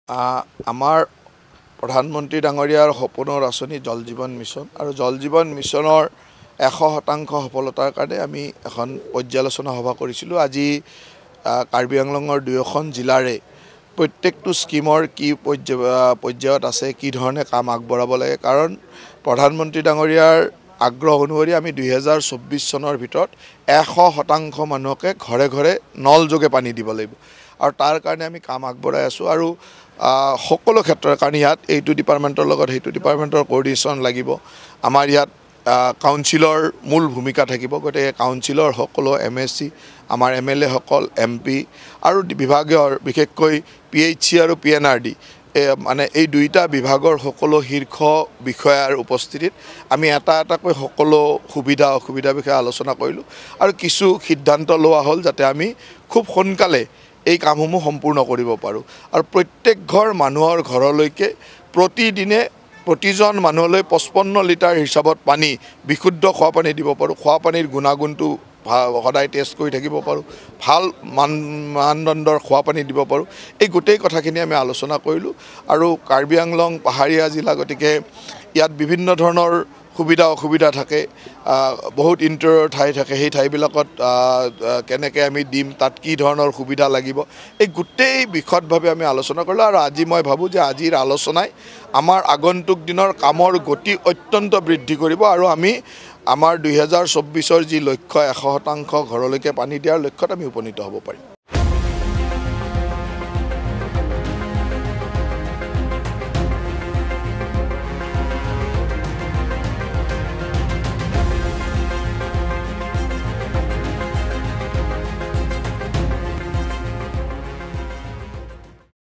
'২০২৪ চনৰ ভিতৰত সকলোৱে ঘৰে-ঘৰে লাভ কৰিব পানী' : জয়ন্ত মল্ল বৰুৱা
▶️ '২০২৪ চনৰ ভিতৰত সকলোৱে ঘৰে-ঘৰে লাভ কৰিব পানী'
▶️ ডিফুৰ জনস্বাস্থ্য কাৰিকৰী আৰু পঞ্চায়ত গ্ৰামোন্নয়নৰ বিষয়াসকলৰ সৈতে পৰ্যালোচনাত সভাত অংশ লৈ জয়ন্ত মল্ল বৰুৱাৰ মন্তব্য